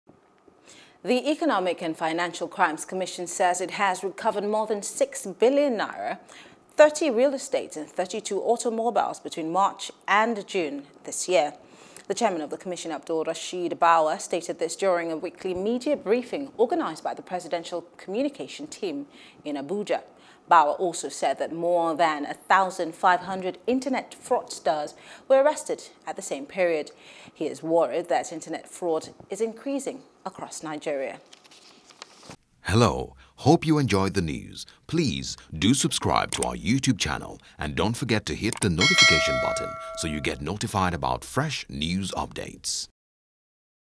EFCC Recovers N6BN And 30 Real Estates In 3 Months - Bawa | NEWS
The Economic and Financial Crimes Commission says it recovered more than six billion naira, 30 real estates and 32 automobiles between March and June this year.